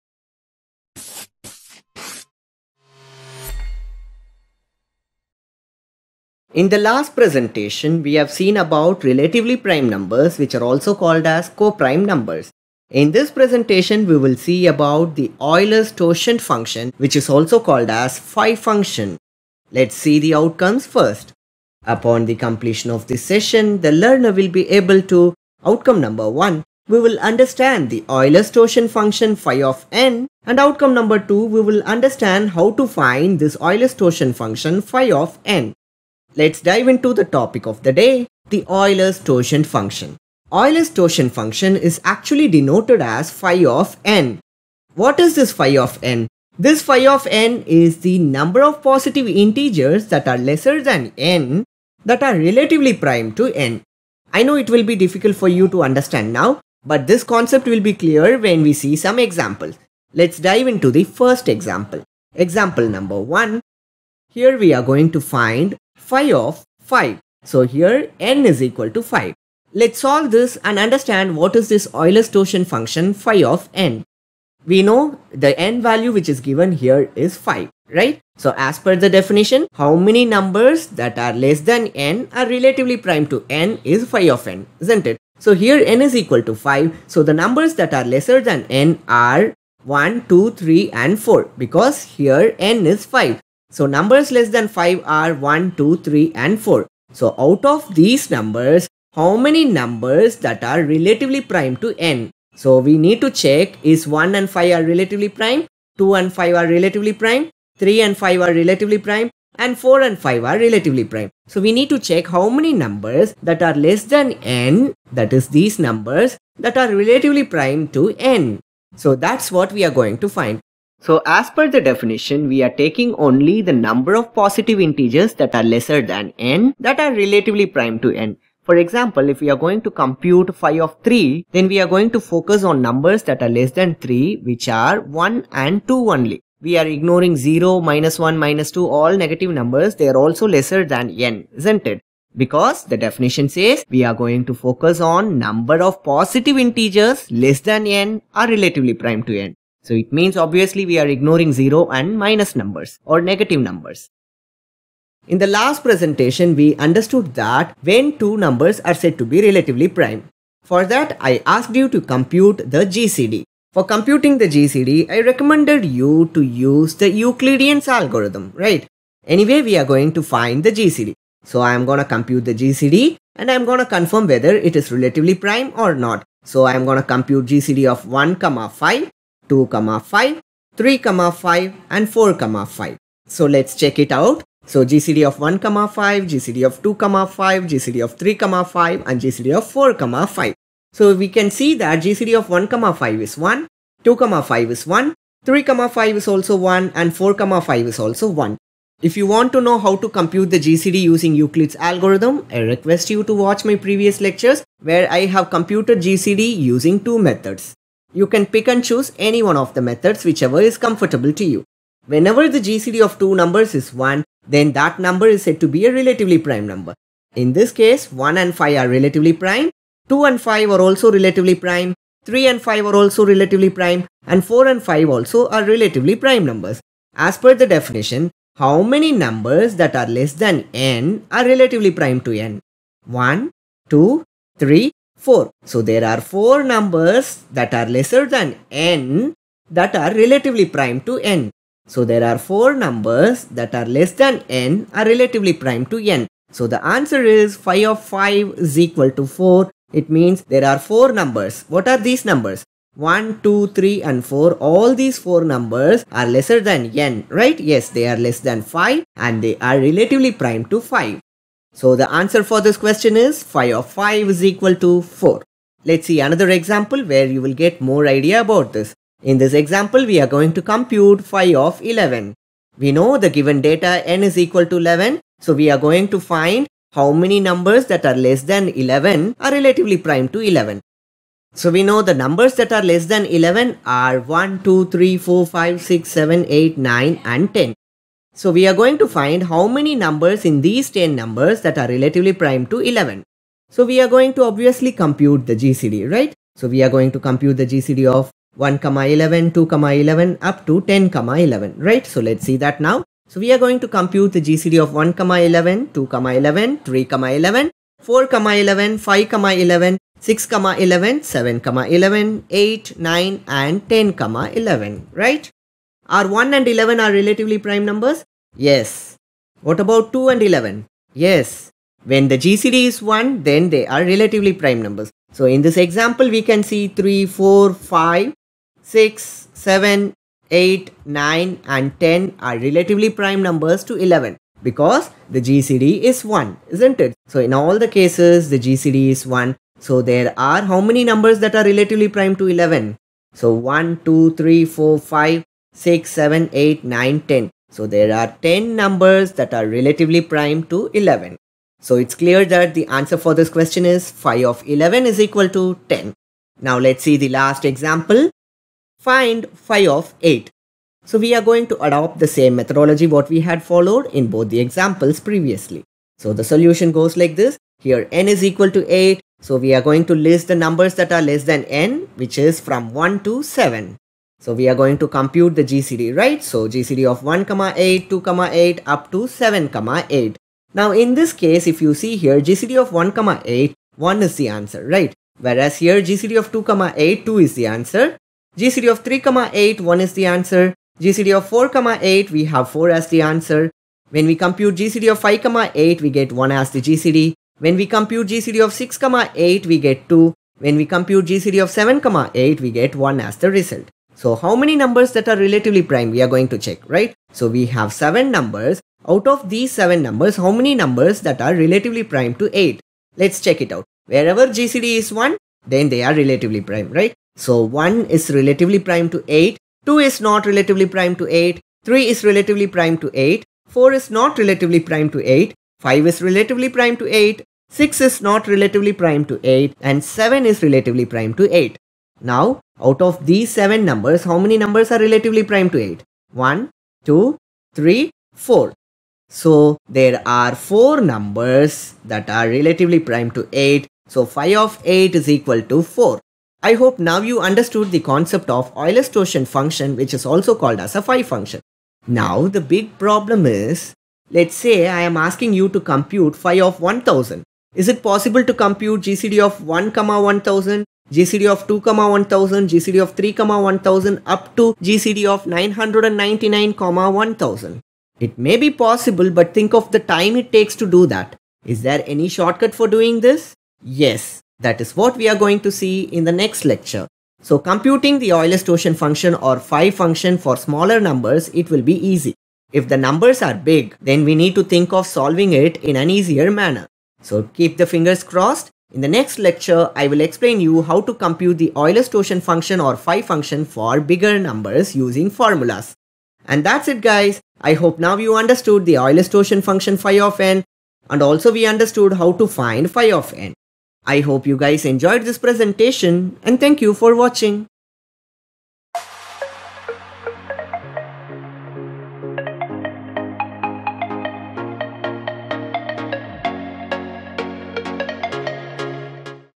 Euler’s Totient Function (Phi Function)
Network Security: Euler’s Totient Function (Phi Function)
Topics Discussed:
1) Definition of Euler’s Totient Function Ф(n) or Phi Function Phi(n).
2) Explanation on how to find the Ф(n).
3) Solved examples of finding Ф(n) for smaller numbers.